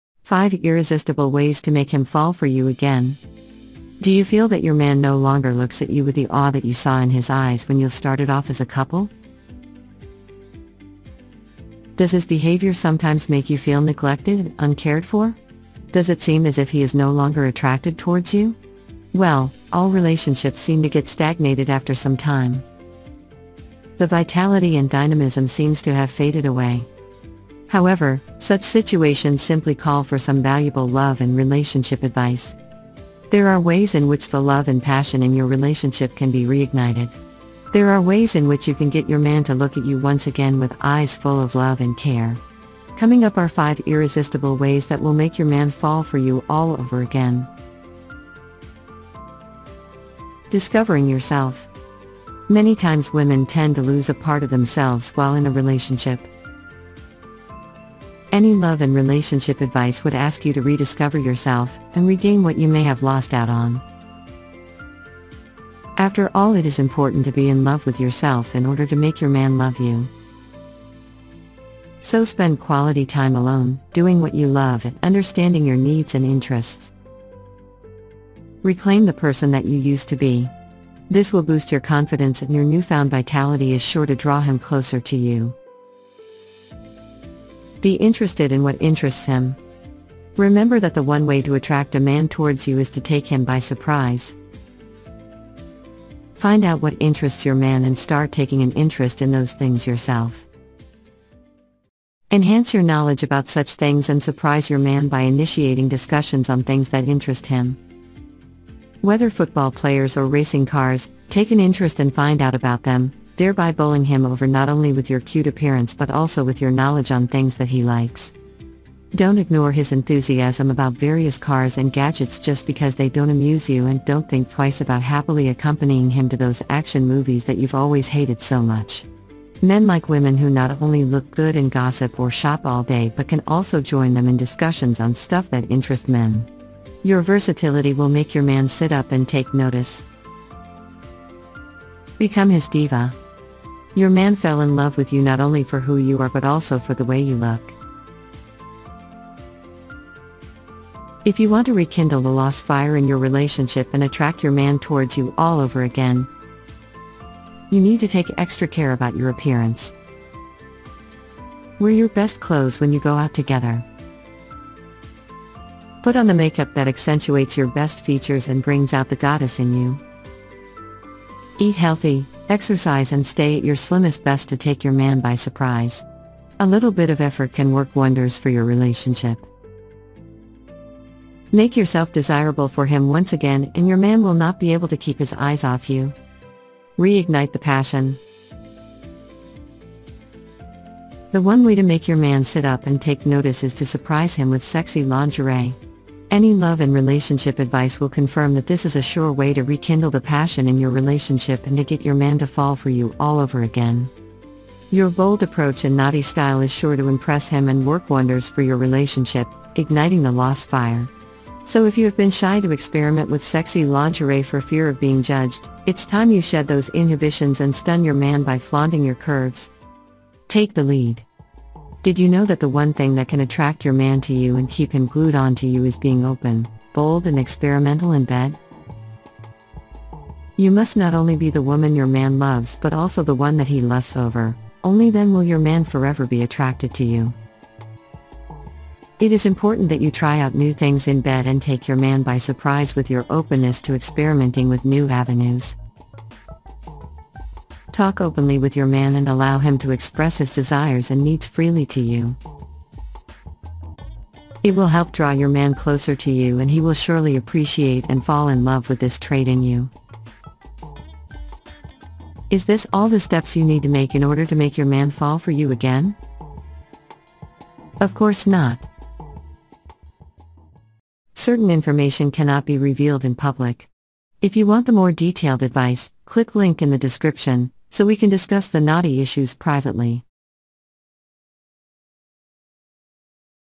5 Ways to Make Him Fall for You Again
Do you feel that your man no longer looks at you with the awe that you saw in his eyes when you'll started off as a couple? Does his behavior sometimes make you feel neglected and uncared for? Does it seem as if he is no longer attracted towards you? Well, all relationships seem to get stagnated after some time. The vitality and dynamism seems to have faded away!
However, such situations simply call for some valuable love and relationship advice. There are ways in which the love and passion in your relationship can be reignited! There are ways in which you can get your man to look at you once again with eyes full of love and care! Coming up are 5 irresistible ways that will make your man fall for you all over again!